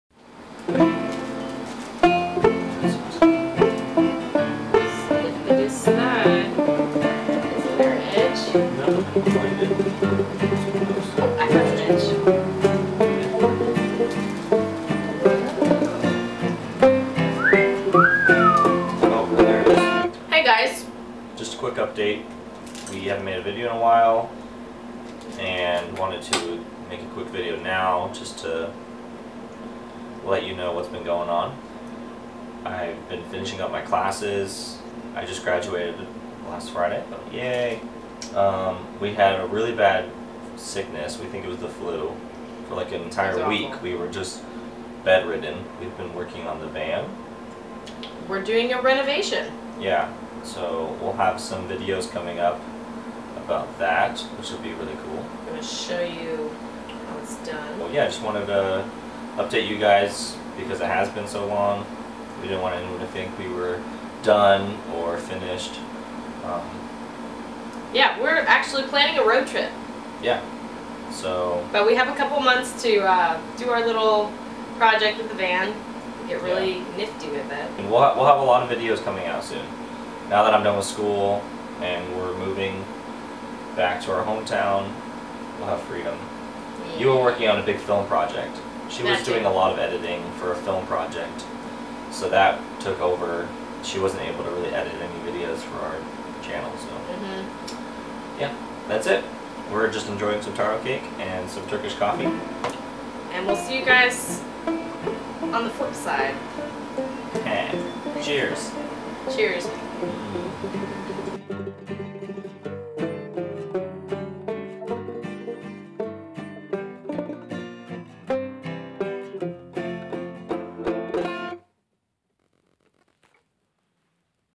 Van Life - May Update!
Sorry for the long break. We had the flu! But now we're feeling better than ever and we want to give you an update.  There are renovations happening!